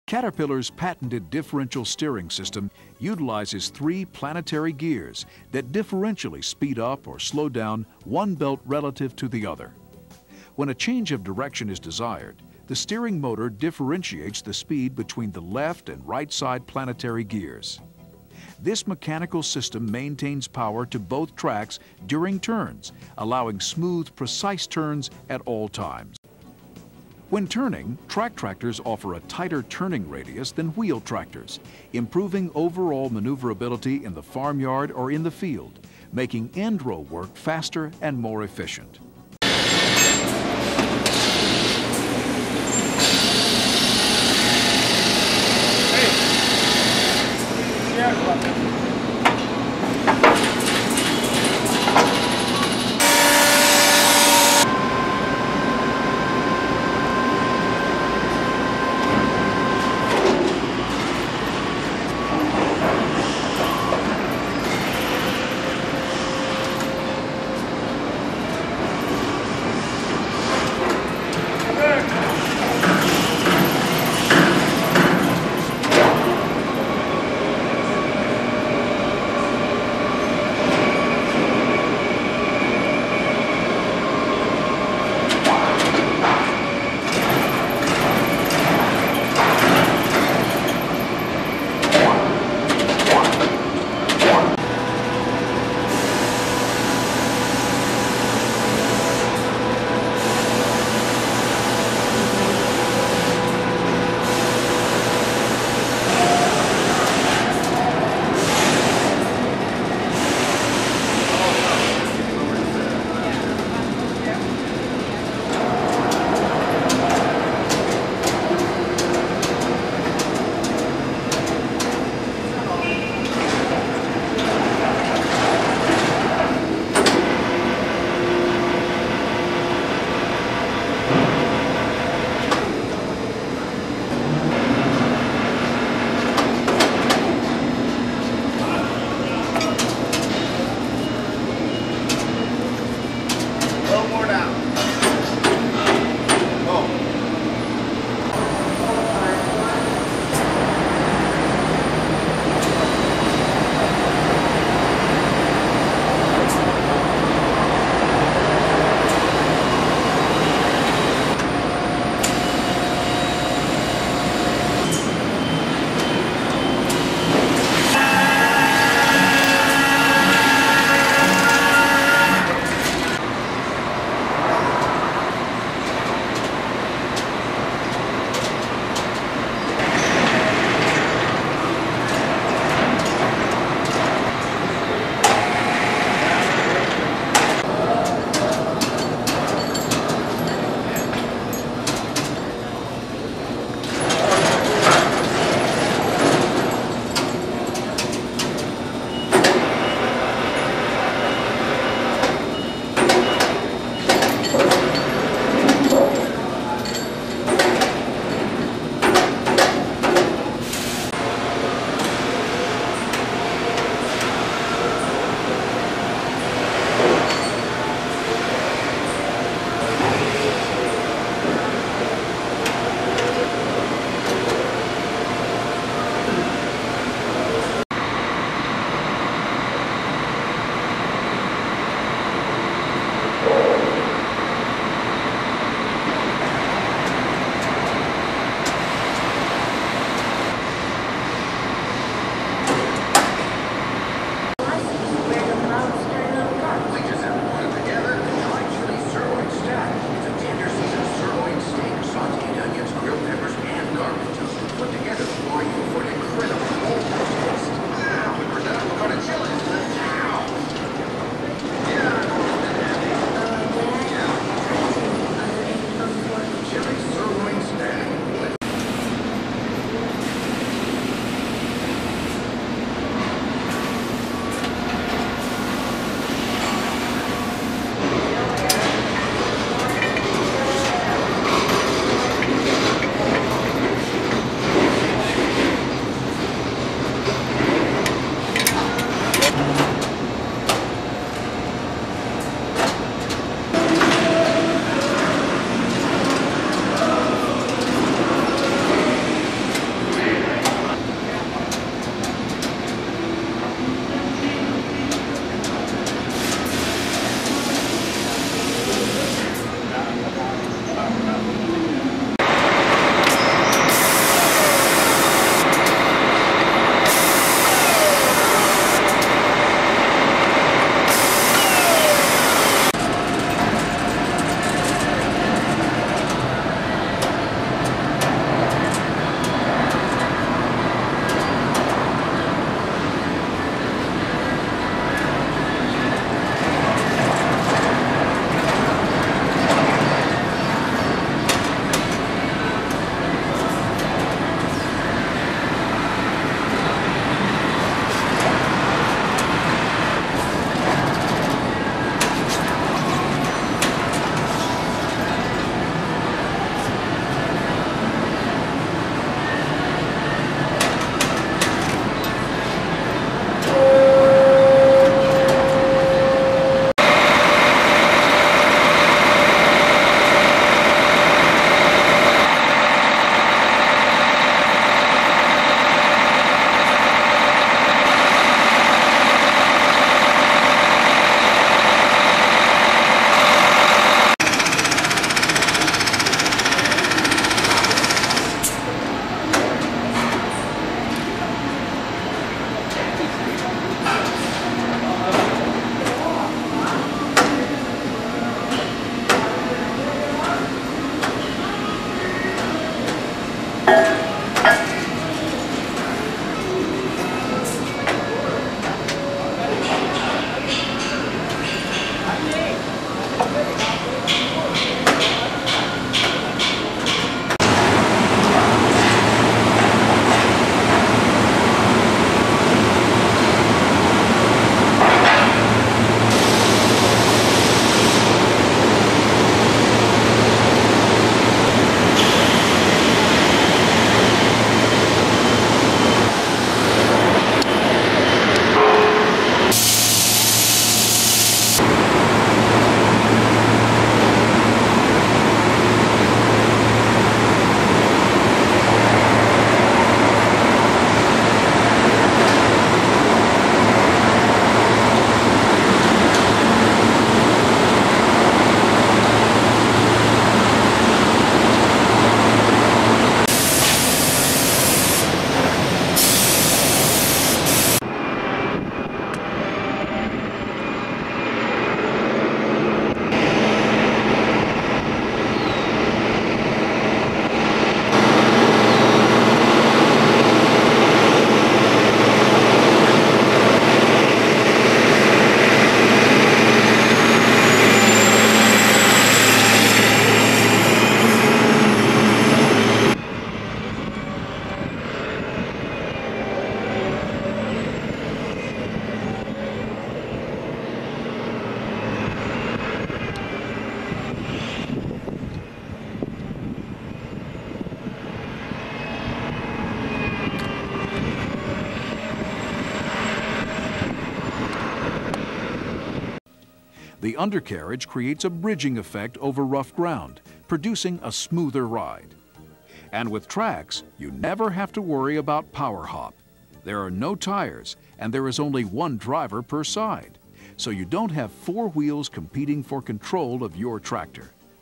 Caterpillar Challenger 95E tractor - production, assembly line, features & testing (video from 2001)
[video: Caterpillar Agricultural Products Inc.]